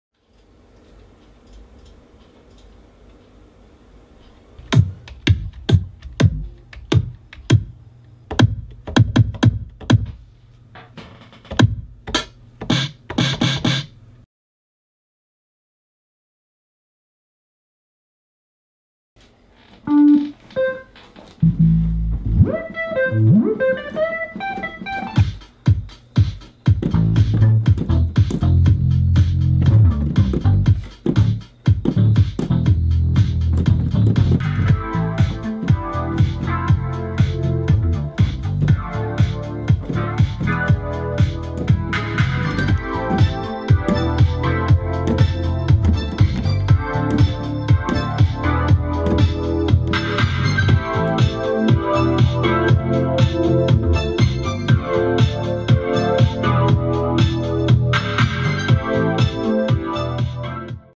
Willow Driver - Making the beat
This is the video documentation that Willow submitted for his HSC Industrial Technology Multimedia Major Project 2017. It demonstrates how he created the beat for his 1980s Disco Music Video.